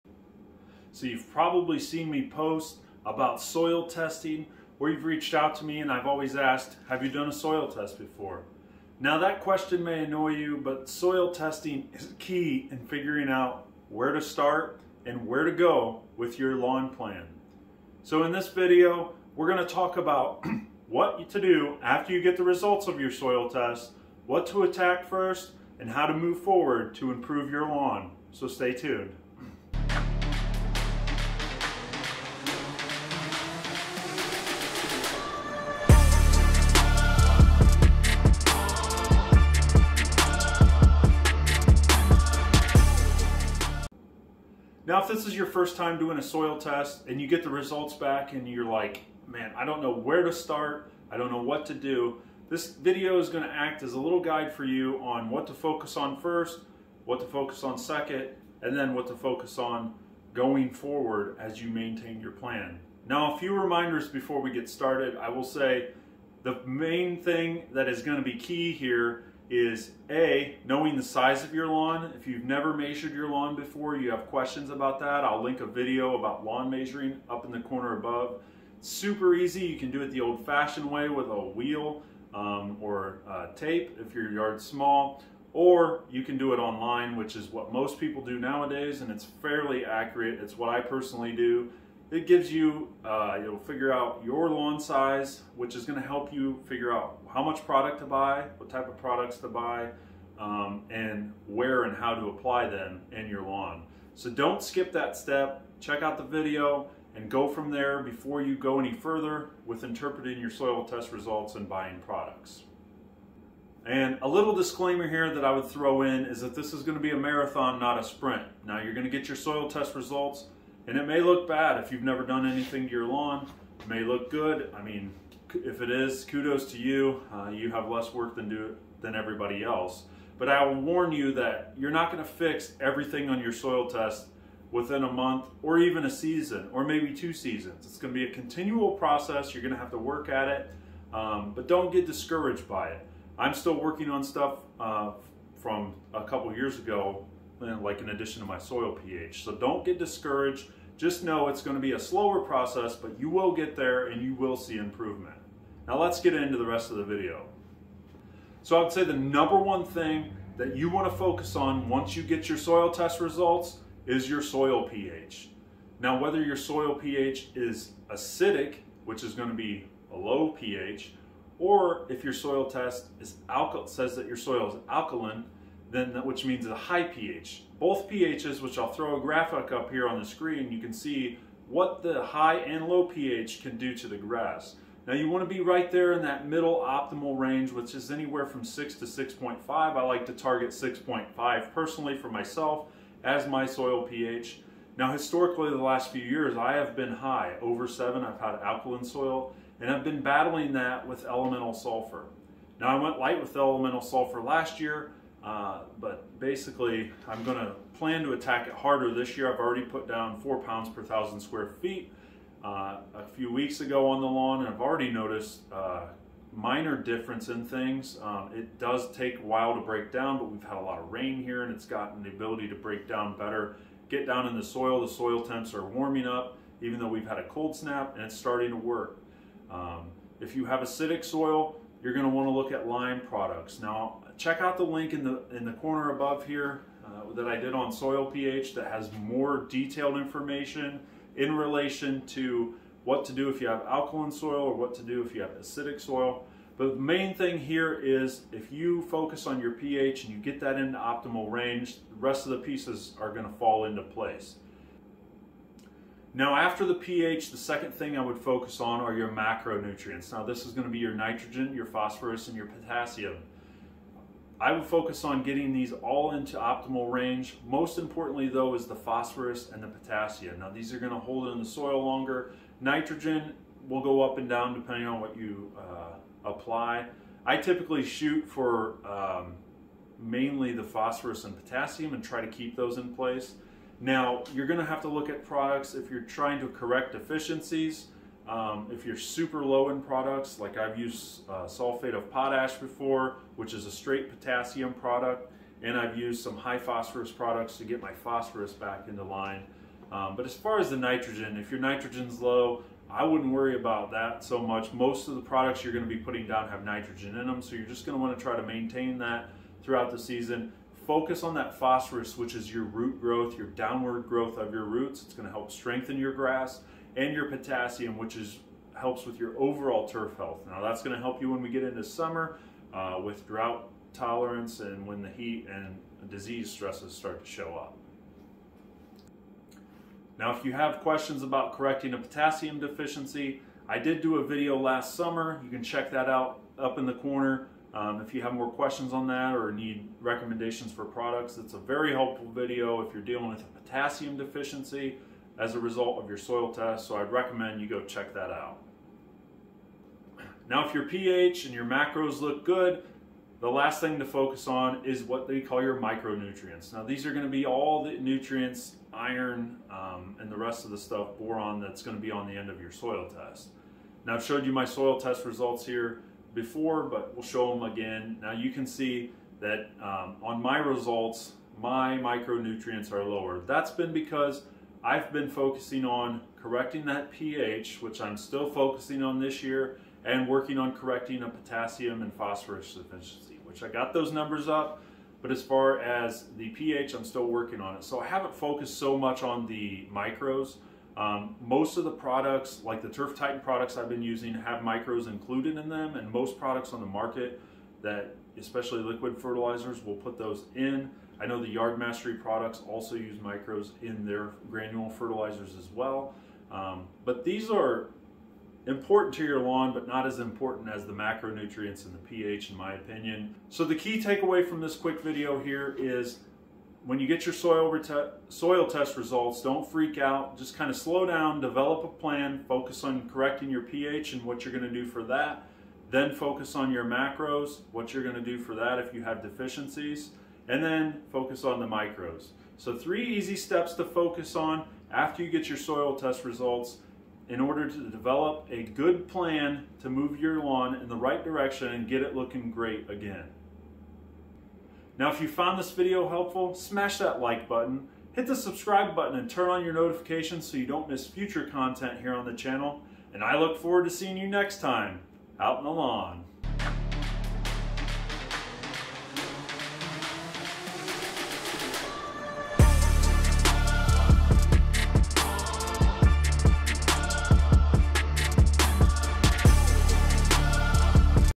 Interpreting Soil Test Results - Achieve A Better Lawn!
So you just did your first soil test and now you have your results, but you are wondering where to begin. If this is you – WELCOME! You are in the right place! In this video, I go over three things to focus on (in order) when you get your soil test results to work on improving your lawn. This process does not have to be confusion, stay calm, develop a plan, and start improving your lawn this season to make the dream lawn you have always wanted reality.

0:00 – Introduction
0:44 – A Few Reminders…
2:38 - #1 Focus on Soil PH
4:14 - #2 Focus on Macros
5:50 - #3 Focus on Micros
6:54 – Wrap Up

Product Links:

Soil Test